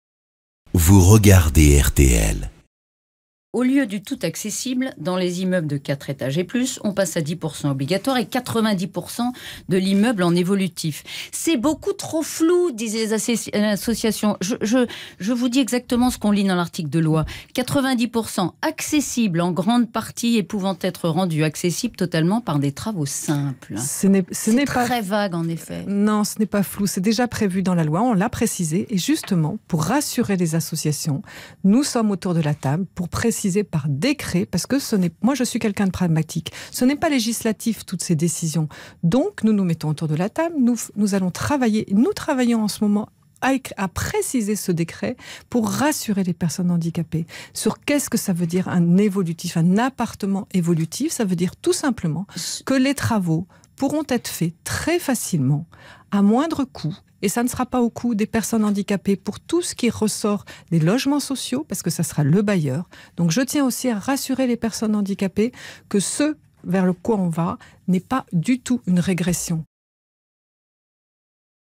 Handicap : Sophie Cluzel annonce un décret sur le caractère "évolutif" du logement
La secrétaire d'État chargée des Personnes handicapées précise le caractère "évolutif" de la loi sur l'accessibilité des logements et annonce la rédaction en cours d'un décret.